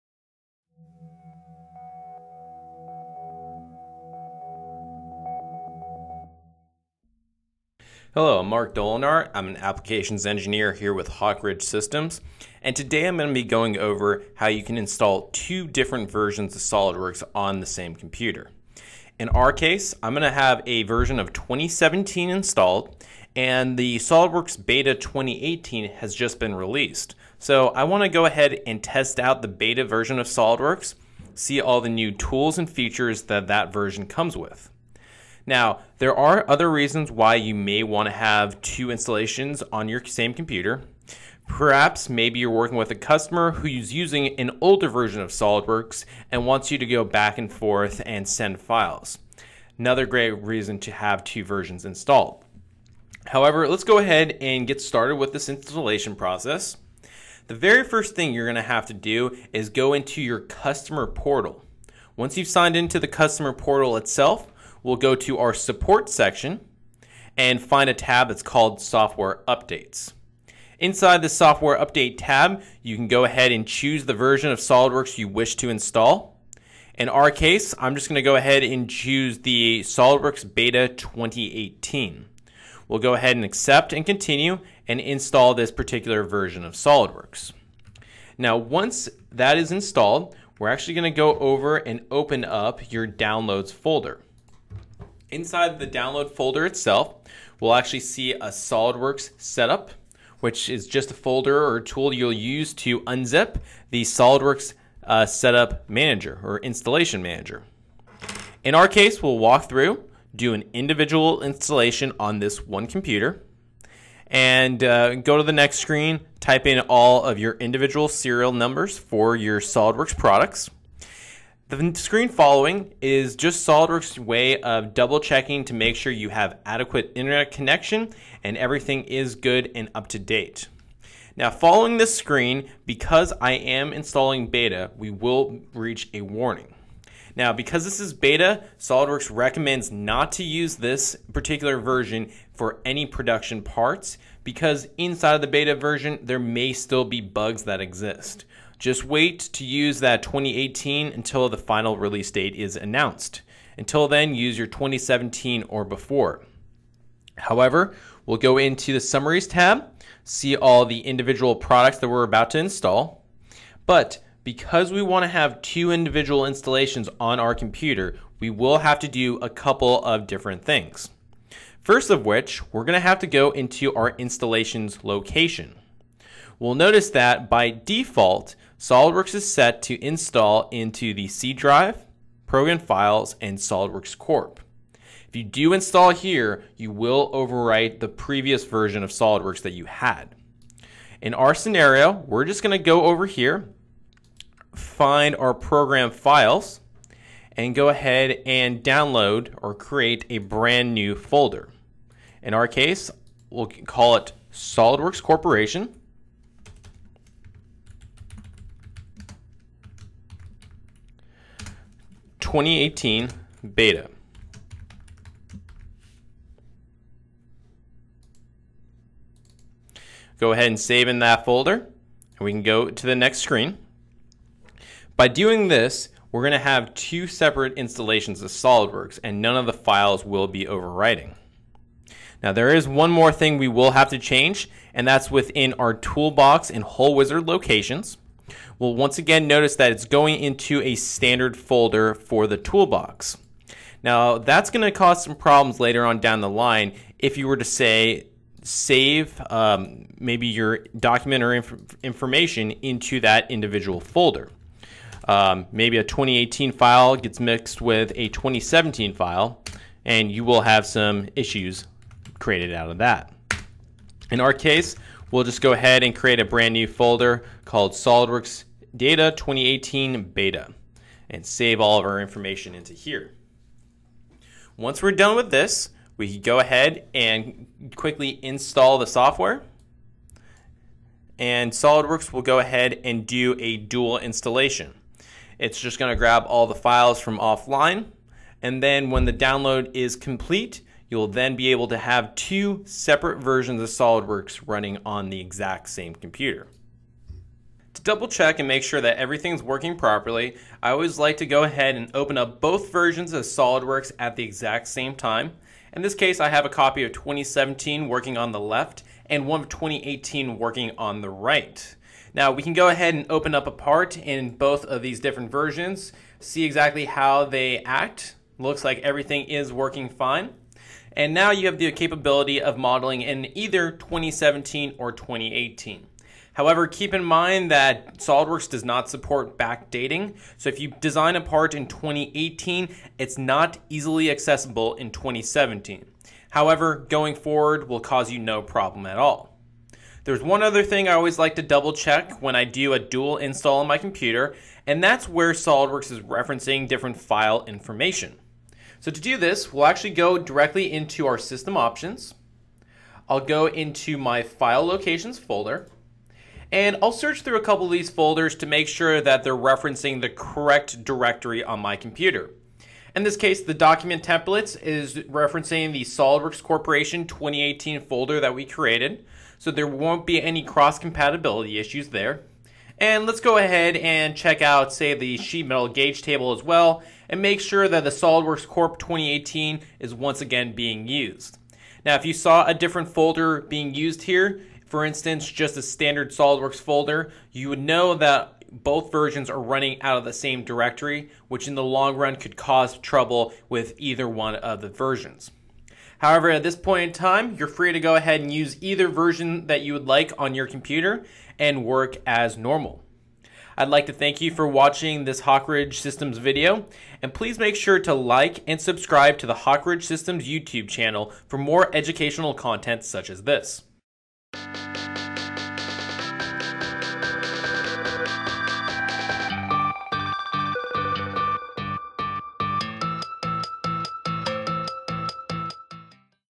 SOLIDWORKS: How to Install Two Separate Releases
It can be advantageous to have multiple versions of SOLIDWORKS installed on a single computer. For instance if you designed in 2017 and a customer used 2016, you would be able to share files freely with this customer. However in our case we want to take a look at the Beta release of SOLIWORKS 2018. Every year SOLIDWORKS puts on a Beta program which allows users to test out it’s upcoming software before it is officially released. Since this is a pre-production version of the software, we need to pay special attention to the installation process as I do not yet want to get rid of my previous installation.